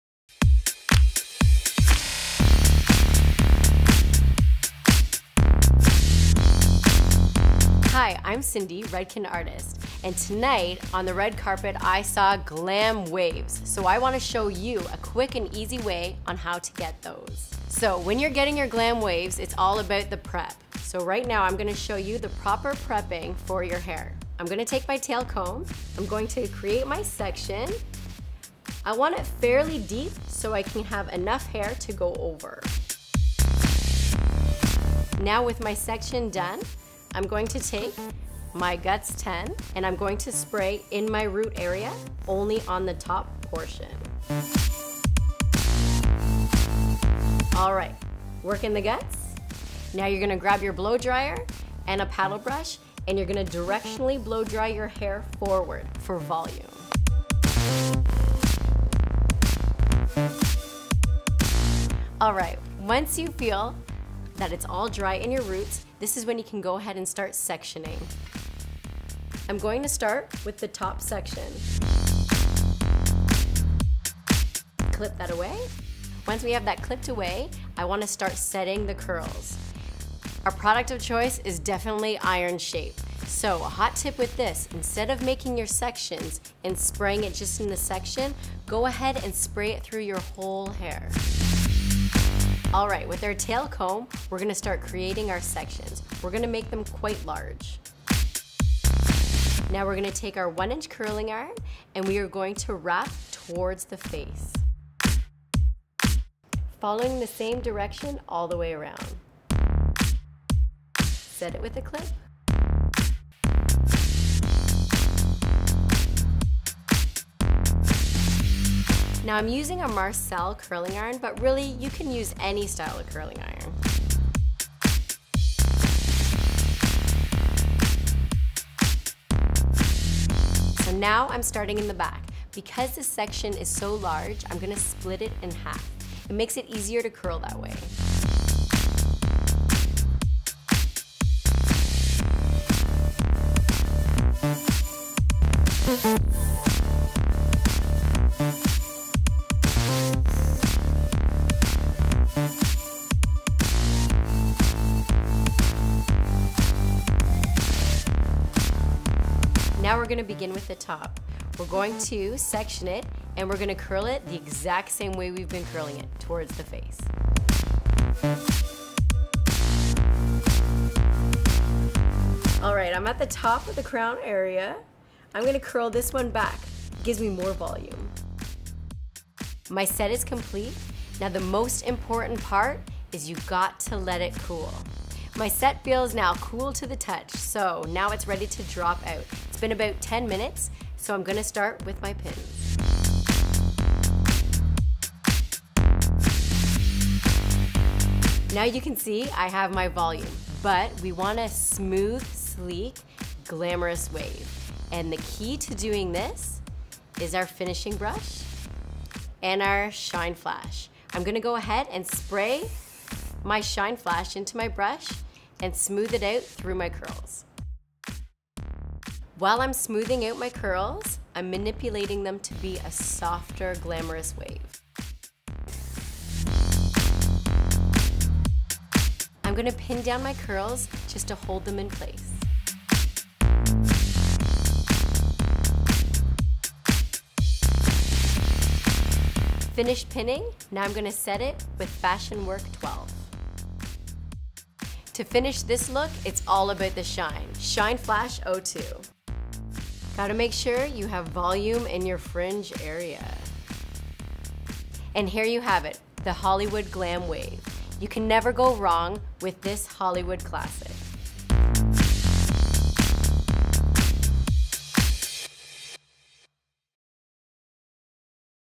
Red Carpet Hair Styling Tutorial: Glamorous Hollywood Waves How To
Hollywood style waves, reminiscent of the 1940's are undoubtedly trending on the red carpet. Glamorous and elegant waves compliment any outfit and look great on just about everyone. In this how to video, Redken Artist Cindy shows you how to use a curling iron to create old Hollywood glamour by setting her hair.

Watch our hairstyle tutorial by Cindy Duplantis and learn how to create glamorous, Hollywood waves from the TV Awards. This is a perfect hair styling tutorial for medium length hair or long hair.